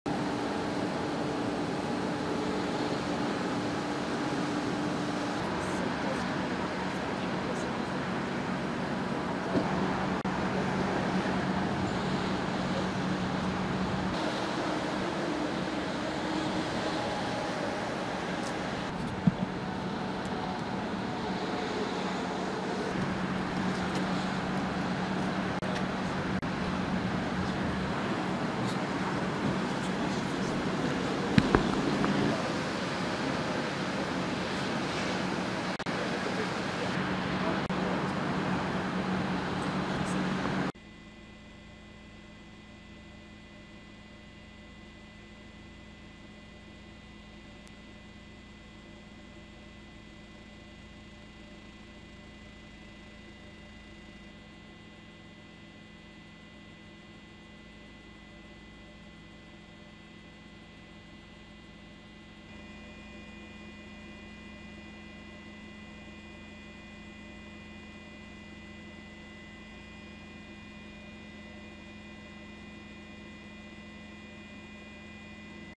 Raw: Deadly Shooting at Mall in Washington State
Authorities in Washington state say five people have been killed during a shooting at a mall north of Seattle. (Sept. 24)